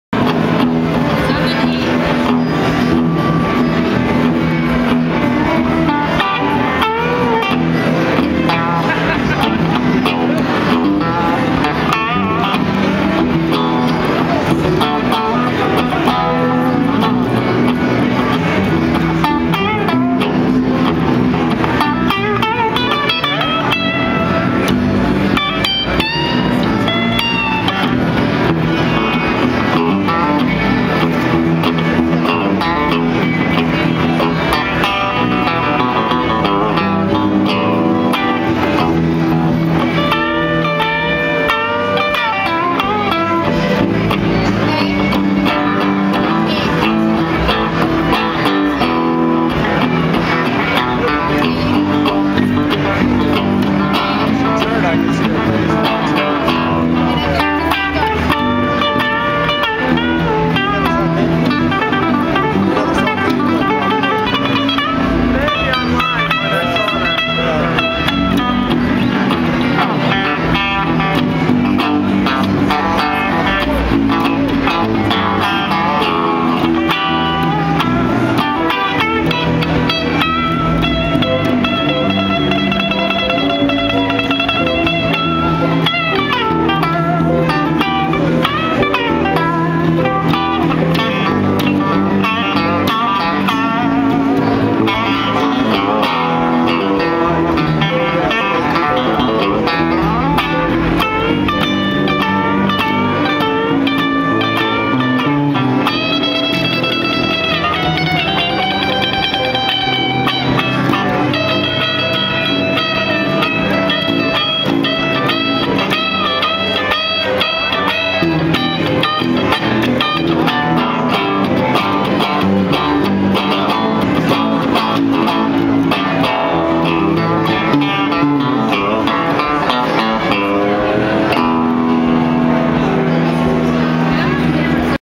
newport-guitar-video.wmv
Katie and Kristi stop by the MAE booth at the Newport Guitar Show in the Hard Rock. April 17, 2010